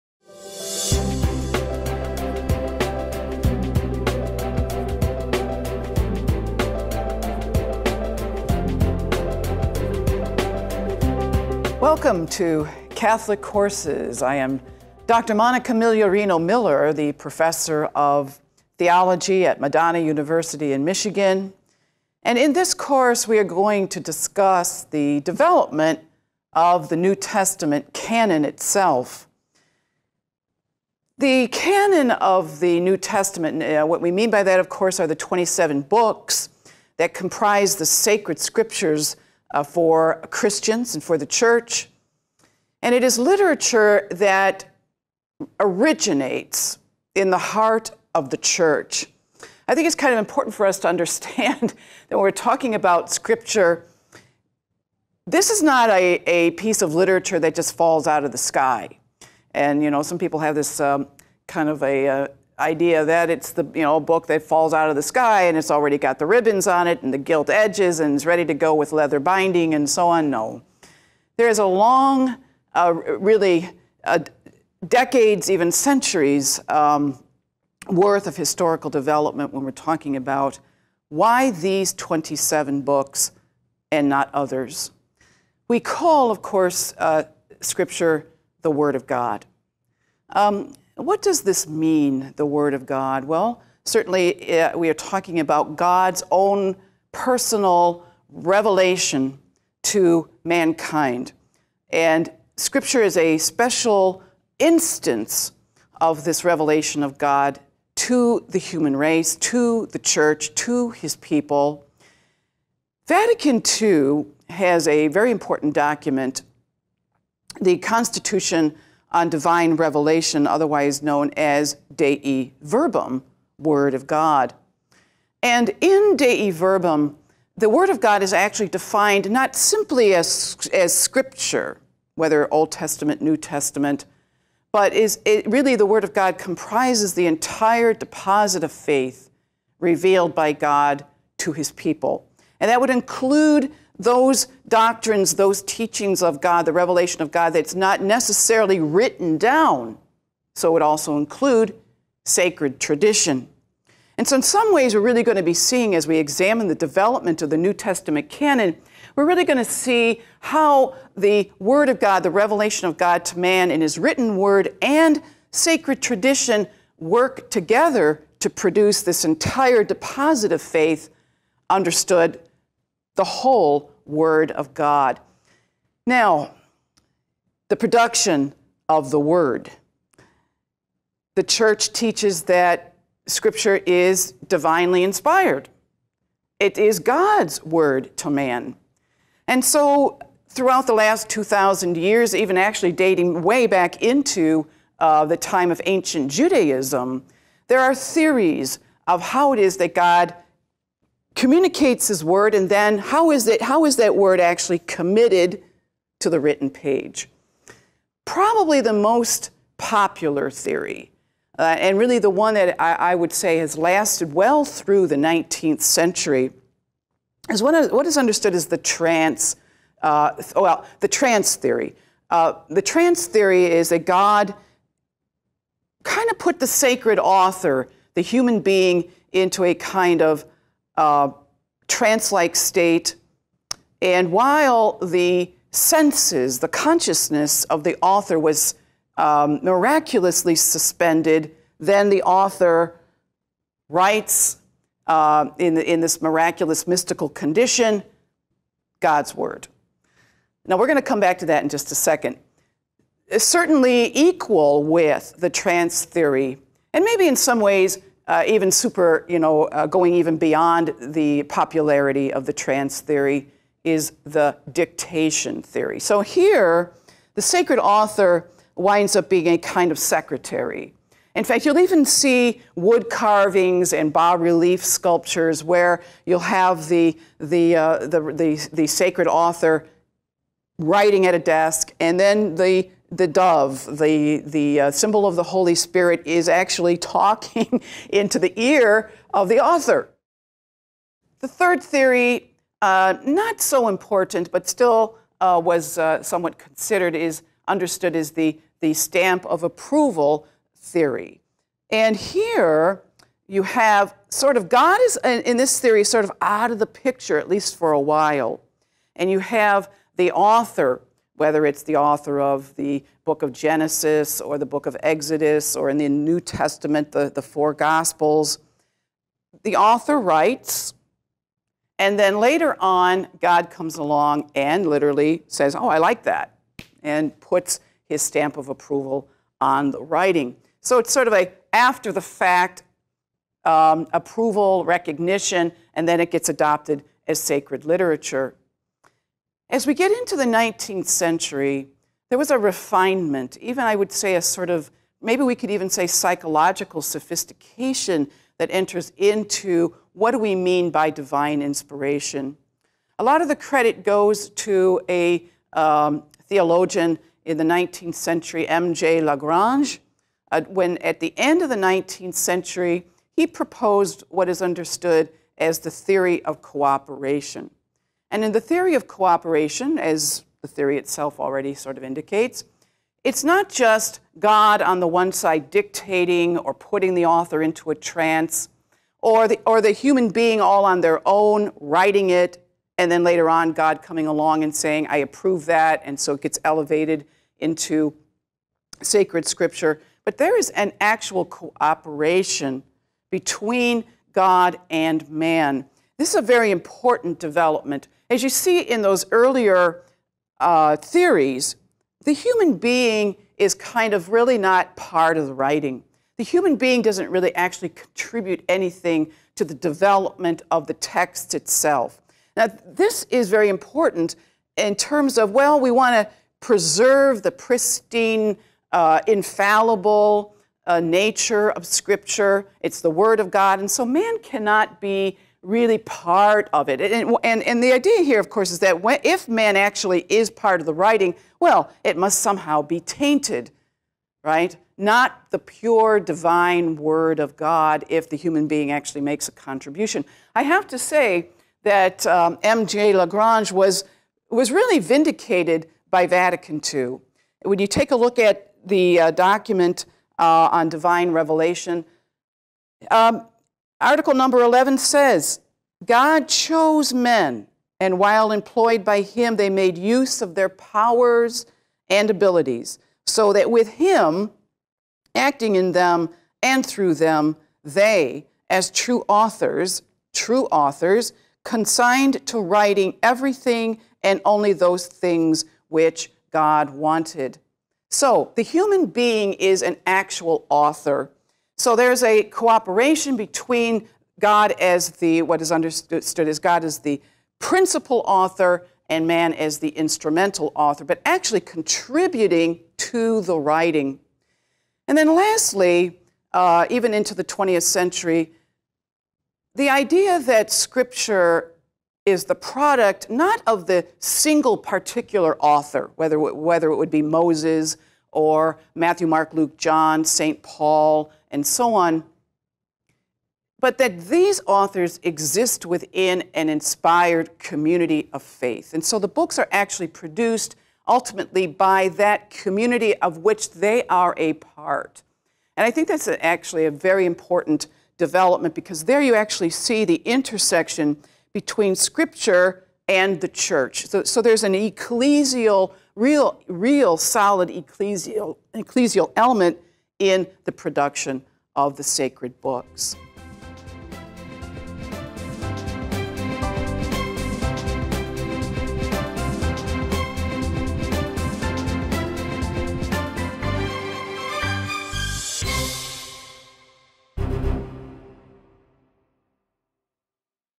New Testament Canon | Monica Migliorino Miller | Lecture 1: A Tour of the New Testament (Excerpt)
Purchase Dr. Monica Migliorino Miller's course on the New Testament Canon

Studying and understanding the New Testament is crucial for the Christian who wishes to deepen their faith. When we take time to better understand the foundations of our faith, we take time to better understand ourselves in the process. Understanding the rich history of the New Testament can only strengthen our faith; it demystifies scripture by making it tangible. We can more confidently profess to believe in Jesus by getting to know those who first professed to believe in Him. Walk through the history of the Christian faith which began with New Testament and continues with us with Dr. Migliorino Miller as your guide.